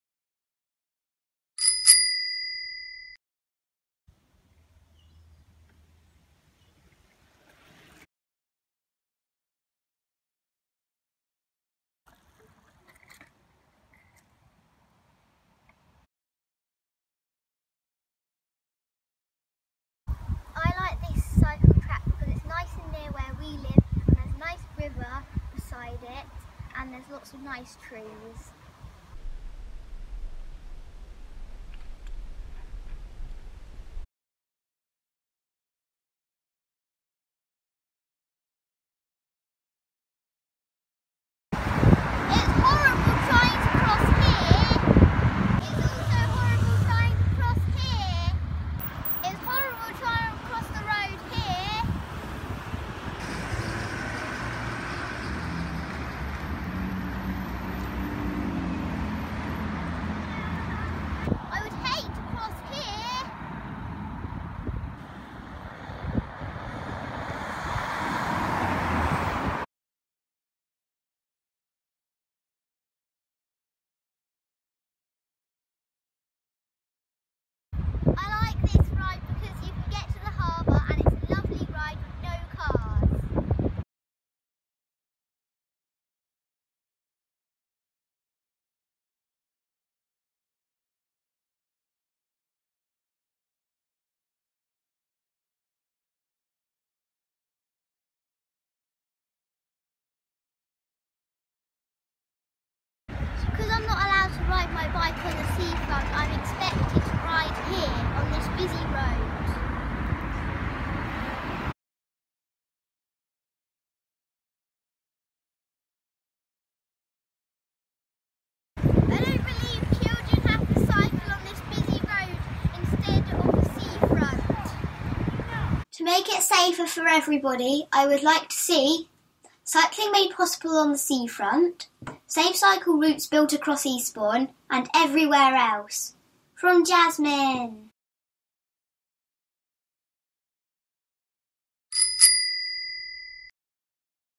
Jasmine's Guide To Eastbourne Cycle Routes
See some of the good and the bad cycle routes in Eastbourne from a child's perspective.